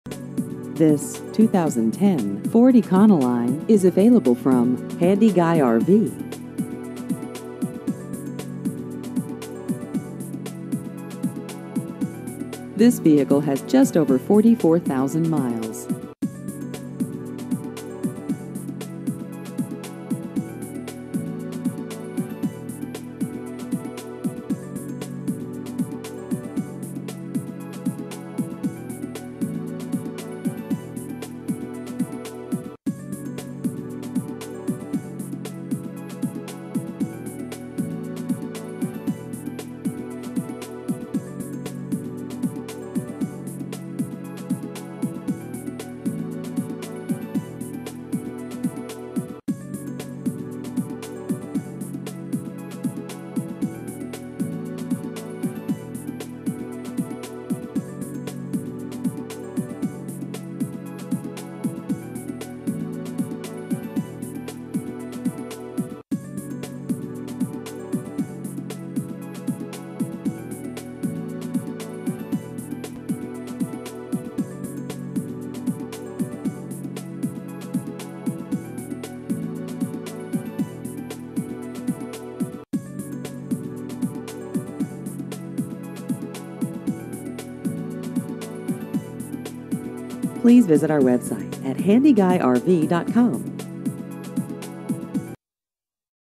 2010 Ford Econoline Used Cars Corona CA
This 2010 Ford Econoline is available from Handy Guy RV.